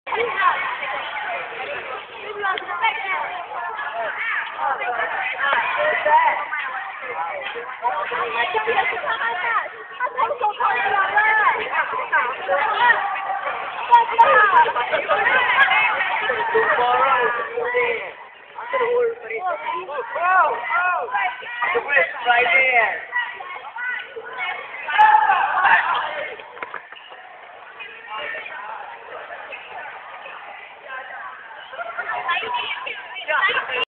030708 WDL UG 3RD OFFICIAL TRAINING - everyone knows dispak!
YAY YAY YAY!! After 2 sessions of vigorious dispak-ing, everyone more or less gets the idea! :)